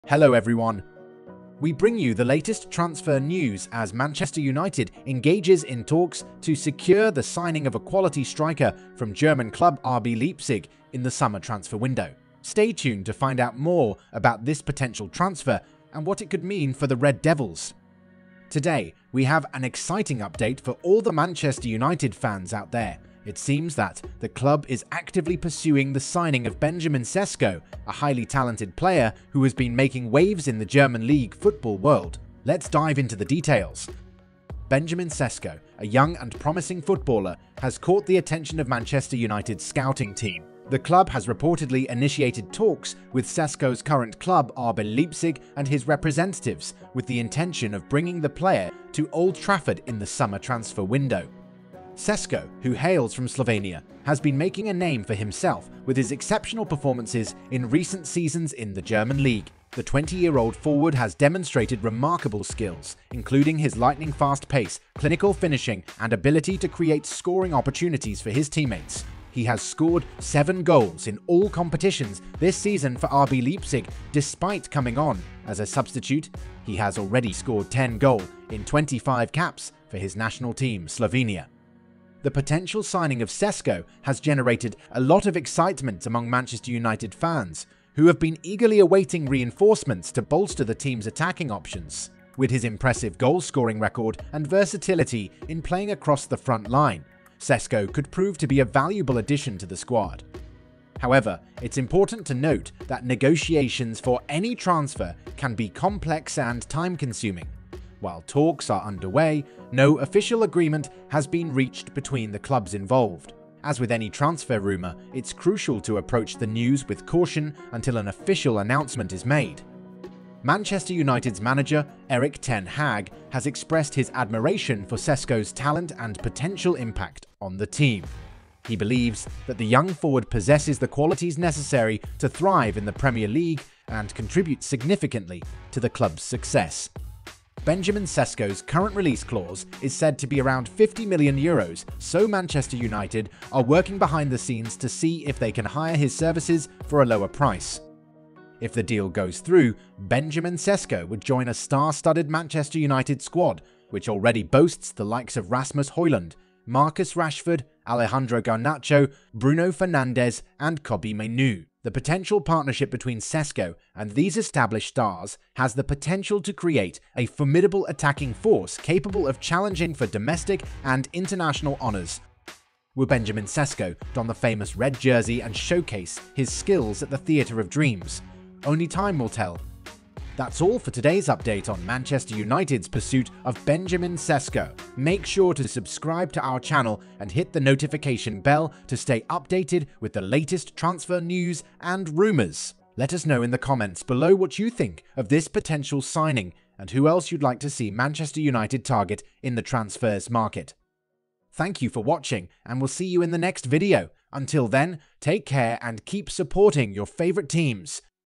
YES🔥Man United finally agreed to a £50m deal to sign a perfect striker to patner Rasmus Hojlund
Today, we have an exciting update for all the Manchester United fans out there. It seems that the club is actively pursuing the signing of Benjamin Sesko, a highly talented player who has been making waves in the German league football world. Let's dive into the details!
Benjamin Sesko, a young and promising footballer, has caught the attention of Manchester United's scouting team. The club has reportedly initiated talks with Sesko's current club RB Leipzig and his representatives, with the intention of bringing the player to Old Trafford in the Summer transfer window.
Sesko, who hails from Slovenia , has been making a name for himself with his exceptional performances in recent seasons in the German league. The 20-year-old forward has demonstrated remarkable skills, including his lightning-fast pace, clinical finishing, and ability to create scoring opportunities for his teammates. He has scored 7 goals in all competitions for RB Leipzig despite coming on as a substitute, He has already scored 10 goal in 25 caps for his national team Slovenia.
The potential signing of Sesko has generated a lot of excitement among Manchester United fans, who have been eagerly awaiting reinforcements to bolster the team's attacking options. With his impressive goal-scoring record and versatility in playing across the front line, Sesko could prove to be a valuable addition to the squad.
However, it's important to note that negotiations for any transfer can be complex and time-consuming. While talks are underway, no official agreement has been reached between the clubs involved. As with any transfer rumor, it's crucial to approach the news with caution until an official announcement is made.